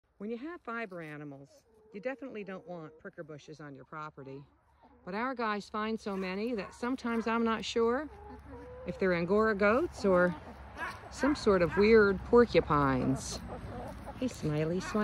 Angora goat or porcupine? You decide!
Short video of my favorite goat 'Smiley' after she's been grazing on some prickly pricker bushes. These are a delicacy (along with poison ivy, oak and sumac!) grazing greenpasture Love these goats. Farm living is the life for me.